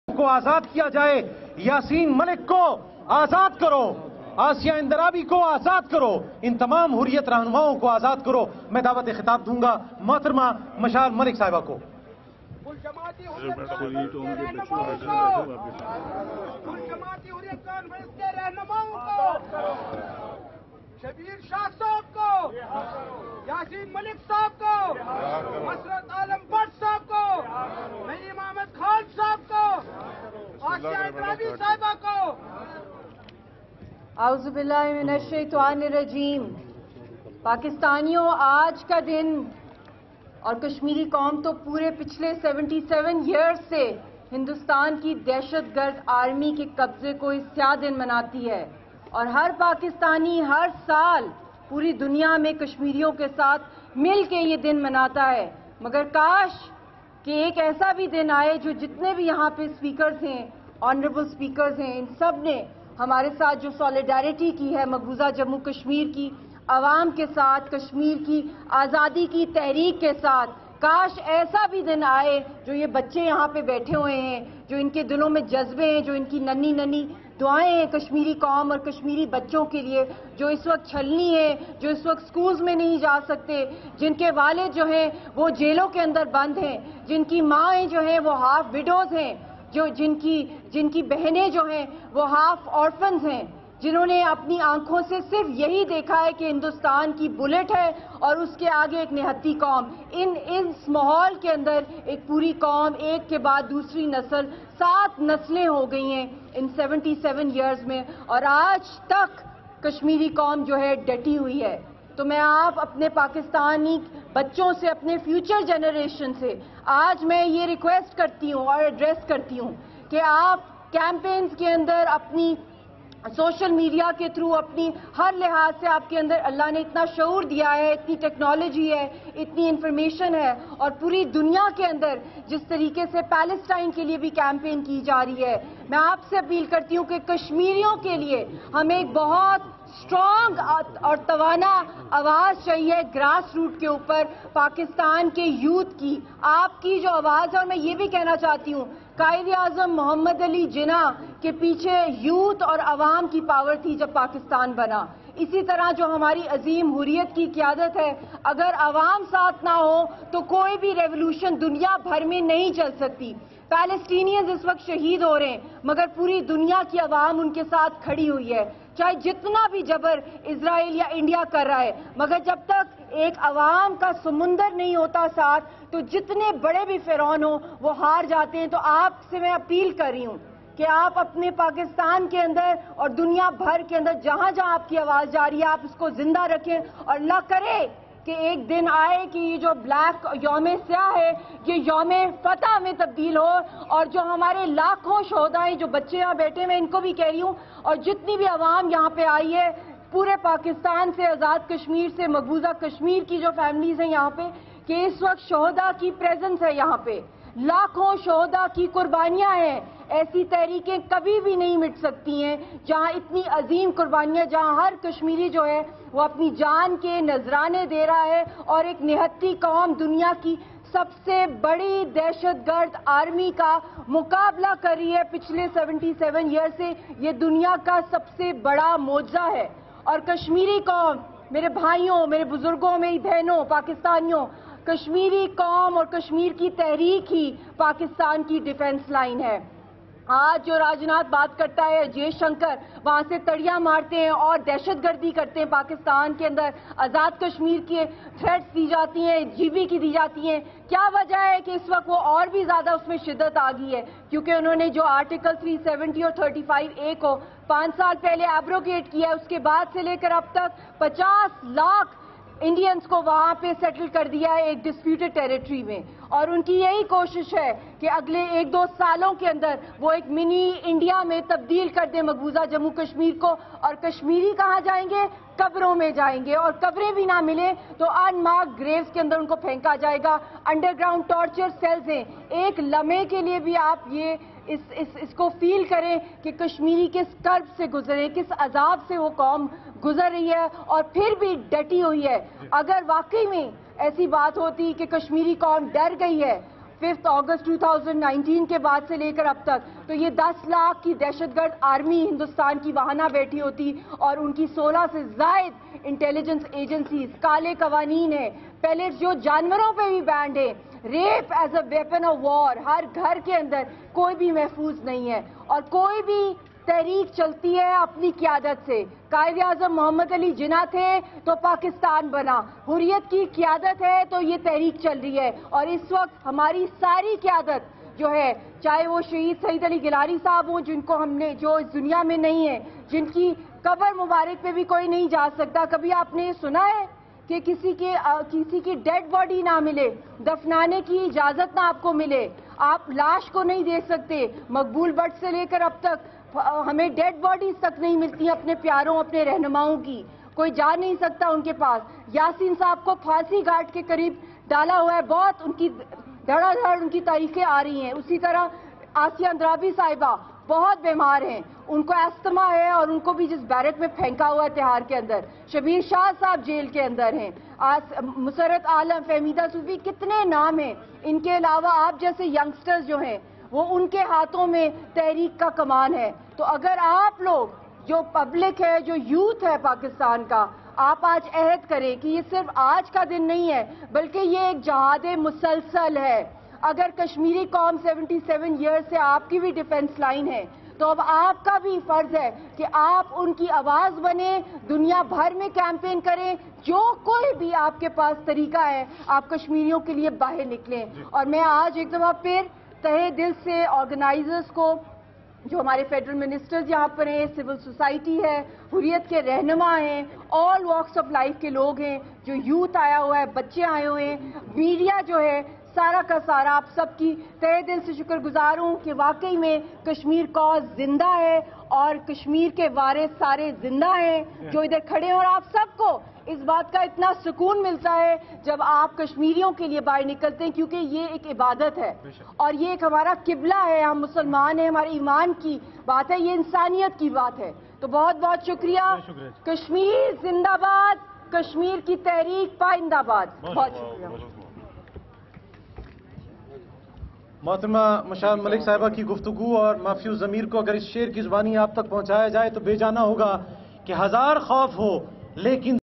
Mushaal Hussein Mullick's Powerful Speech on Kashmir Black Day | D Chowk, Islamabad - 27th Oct 2024
Mushaal Hussein Mullick Chairperson Peace and Culture Organisation and Wife of Jailed Kashmiri Hurriyat Leader Mohammad Yasin Malik addressing a public gathering at D Chowkh Islamabad today on Kashmir Black Day 27th October